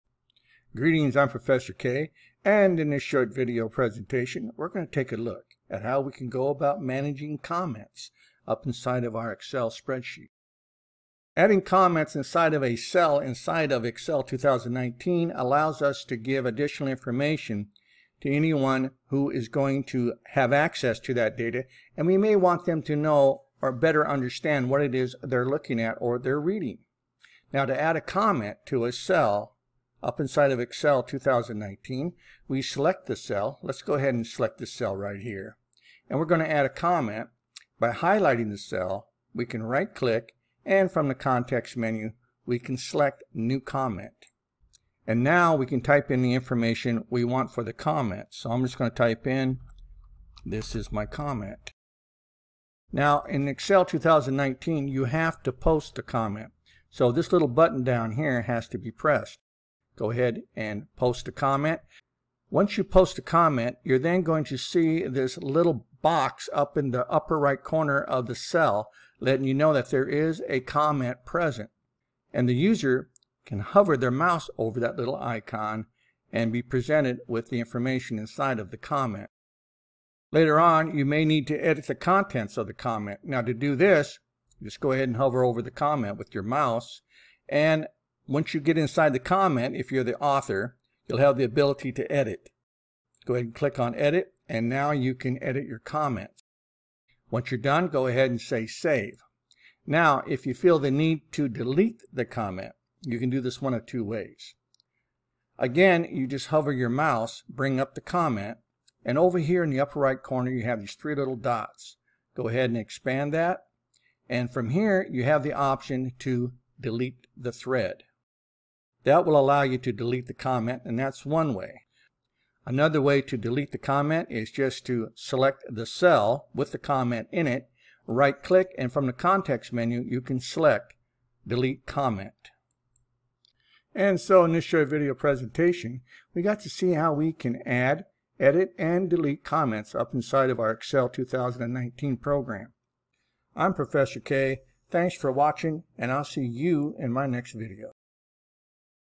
Excel 2019 - How to use and manage comments
In this short video, you will learn how to add, edit and delete comments in Excel 2019.